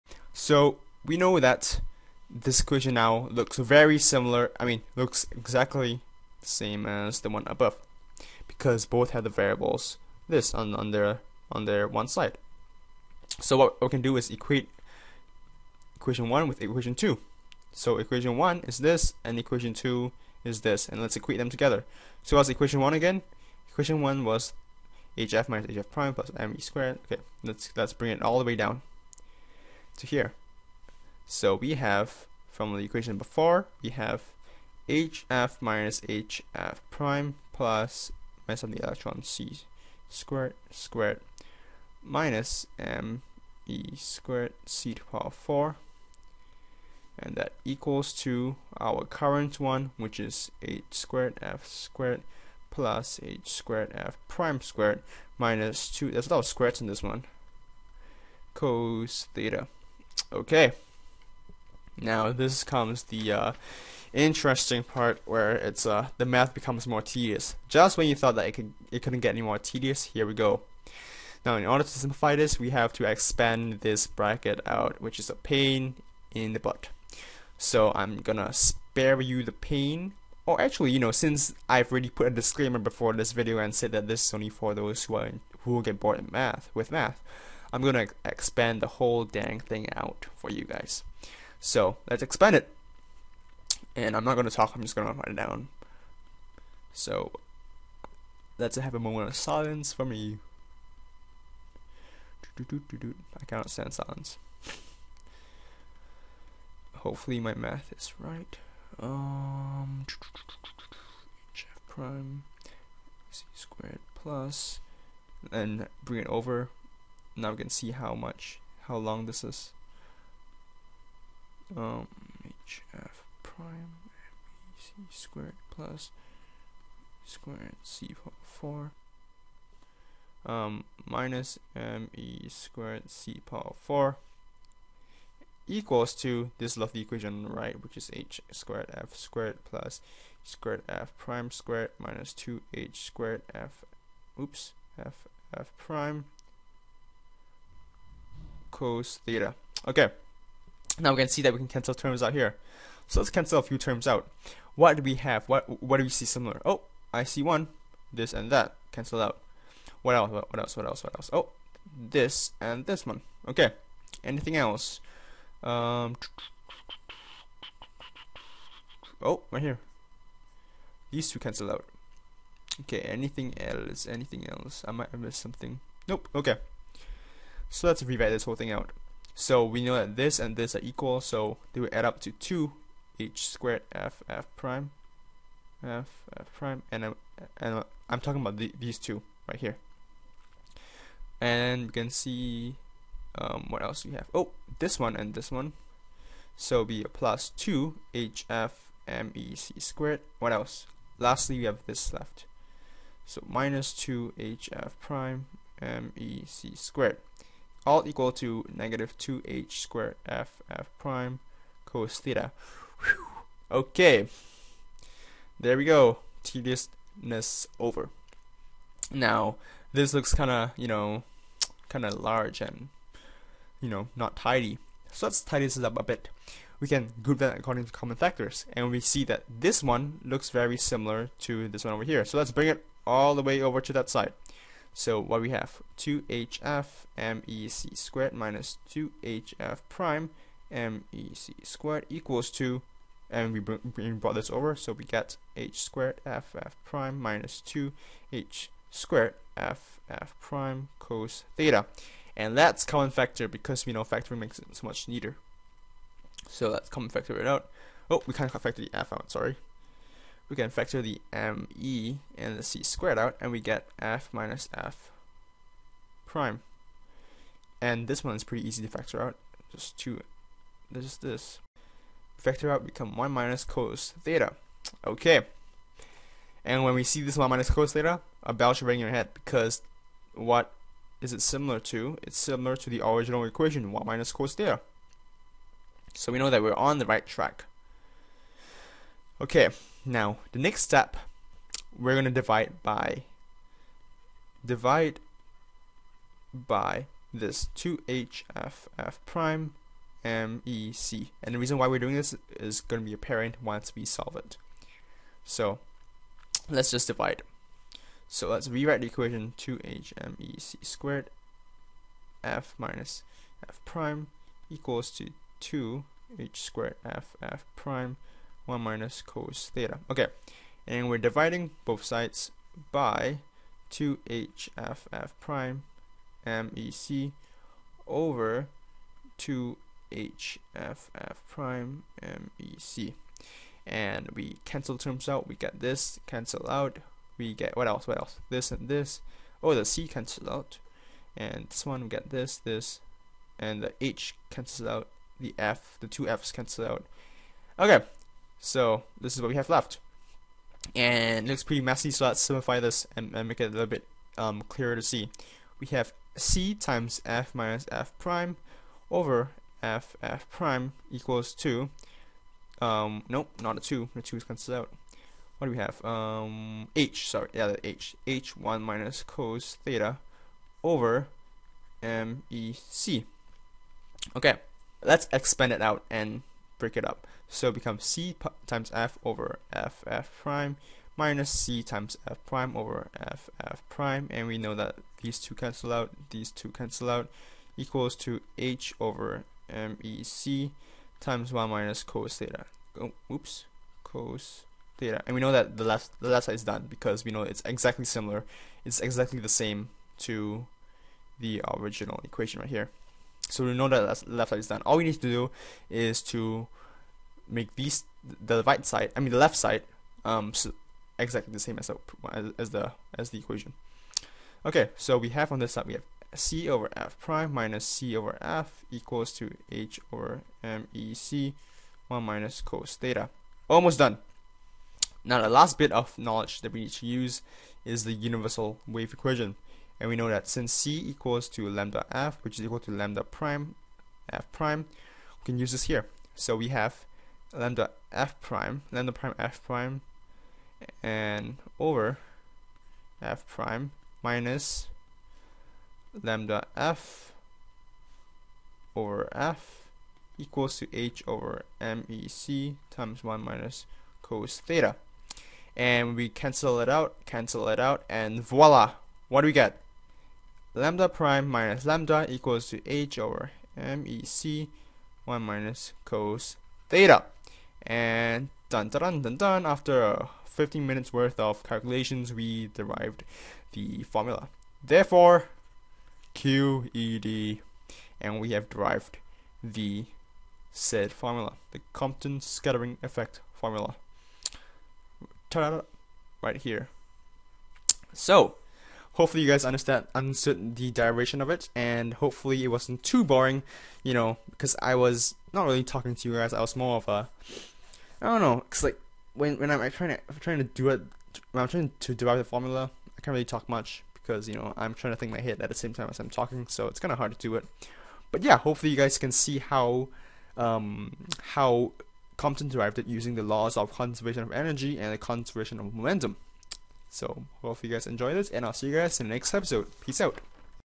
Deriving the Compton Scattering Formula (Part 3)
This is part three of my lesson on the derivation of the scattering formula relating the shift in the photon's wavelength to its scattering angle.  In this part, I go over the final calculations combining the previous equations to finish off the derivation of the scattering formula.